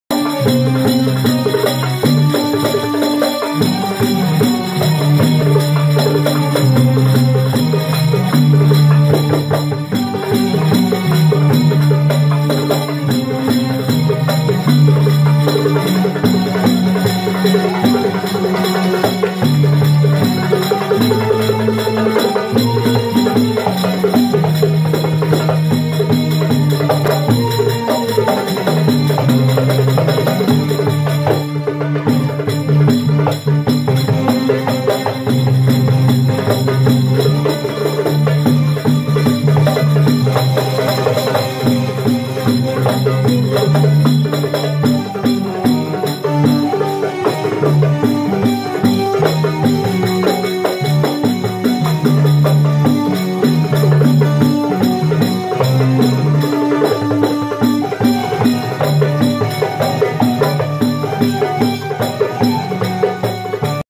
CUÁL ES UNO DE LOS PRINCIPALES Y TÍPICOS SHOWS DE LAS POLLERAS QUÉ OFRECEN LOS CRUCEROS POR EL NILO
IMPRESIONANTE SHOW!!!